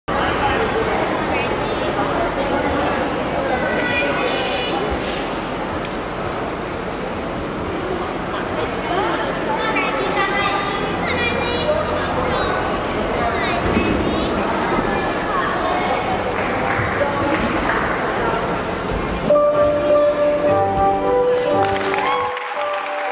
Riley Hochman 1st Bars SoCal East Sectionals 2017 Wildfire Gymnast Level 3
1st Place Bars 9.650. 3rd AA 37.825.